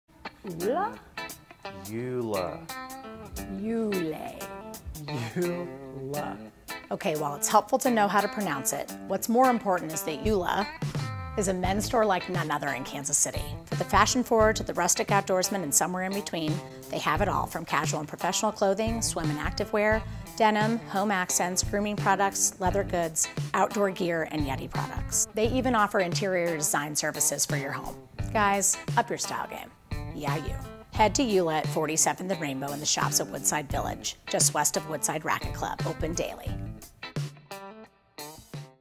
ULAH A Men's Apparel & Lifestyle Store in Kansas City theminivanmom.com
ULAH is one of the coolest men's stores in Kansas City, and offers a thoughtfully curated collection of ready to wear clothing, accessories, gifts, essentials, home accents and custom furniture!

Visit Ulah at 47th and Rainbow in the shops at Woodside.